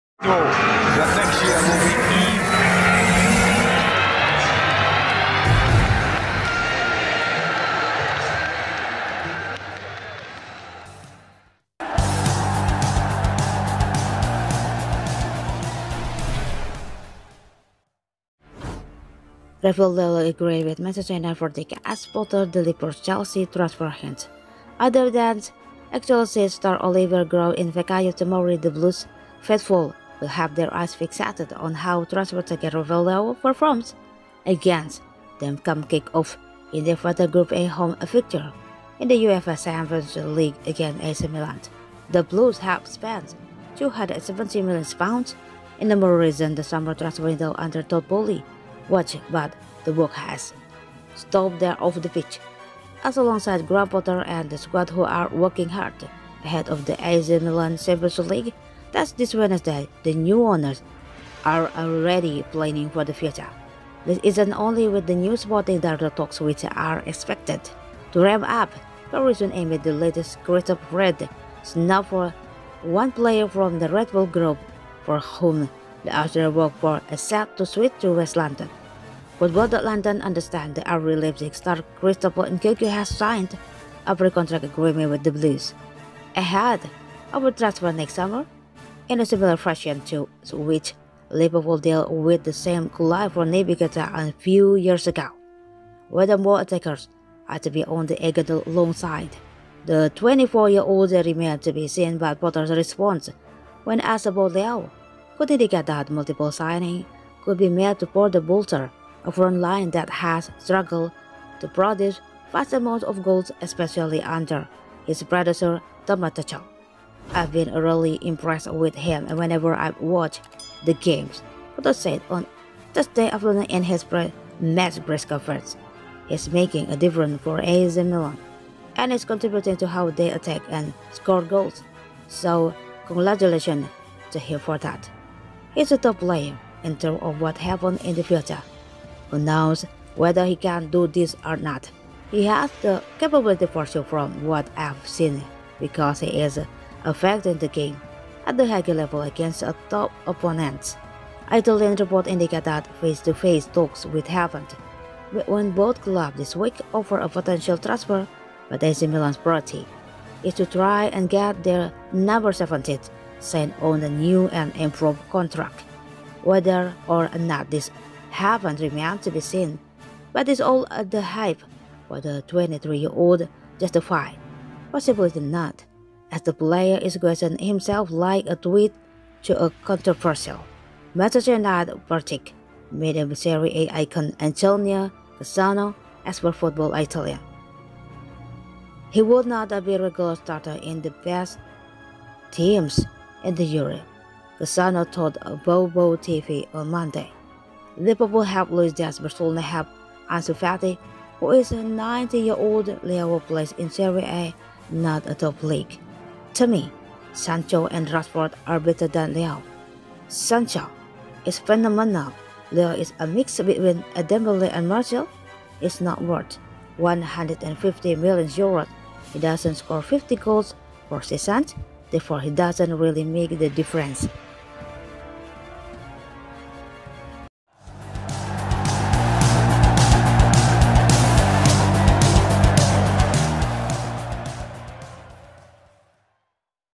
The Portuguese World-Class agrees with Man Utd signing as Potter delivers Chelsea transfer hint
Rafael Leao agrees with Man Utd verdict as Potter delivers Chelsea transfer hint

Other than ex-Chelsea stars Olivier Giroud and Fikayo Tomori, the Blues faithful will have their eyes fixated on how transfer target Rafael Leaoperforms against them come kick-off in their vital Group E home fixture in the UEFA Champions League against AC Milan.

The Blues have spent £270million in the most recent summer transfer window under Todd Boehly's watch but the work hasn't stopped there off the pitch, as alongside Graham Potter and the squad who are working hard ahead of the AC Milan Champions League test this Wednesday, the new owners are already planning for the future.

This isn't only with the new sporting director talks which are expected to ramp up very soon amid the latest Christoph Freund snub but one player from the Red Bull Group for whom the Austrian works for is set to switch to West London.

football.london understands that RB Leipzig star Christopher Nkunku has signed a pre-contract agreement with the Blues ahead of a transfer next summer, in a similar fashion to which Liverpool dealt with the same club for Naby Keita a few years ago.

Whether more attackers are to be on the agenda alongside the 24-year-old remains to be seen but Potter's response, when asked about Leao, could indicate that multiple signings could be made to further bolster a frontline that has struggled to produce vast amounts of goals, especially under his predecessor Thomas Tuchel.

“I’ve been really impressed with him whenever I’ve watched the games," Potter said on Tuesday afternoon in his pre-match press conference. "He is making a difference for AC Milan and is contributing to how they attack and score goals. So congratulations to him for that.

"He is a top player. In terms of what happens in the future, who knows whether he can do this or not? He has the capability for sure from what I’ve seen because he is affecting the game at the highest level against top opponents.”

Italian reports indicate that face-to-face talks will happen between both clubs this week over a potential transfer but AC Milan's priority is to try and get their No. 17 signed on a new and improved contract.

Whether or not this happens remains to be seen but is all the hype for the 23-year-old justified? Possibly not, as the player in question himself liked a tweet to a controversial Manchester United verdict made by Serie A icon Antonio Cassano, as per Football Italia.

“He would not be a regular starter in the best teams in Europe,” Cassano told Bobo TV on Monday.

“Liverpool have Luis Diaz. Barcelona have Ansu Fati who is 19-year-old. Leao plays in Serie A, not a top league.

“To me, Sancho and Rashford are better than Leao, Sancho is phenomenal. Leao is a mix between Dembélé and Martial. He is not worth €150m he doesn’t score 50 goals per season, therefore he doesn’t really make the difference.”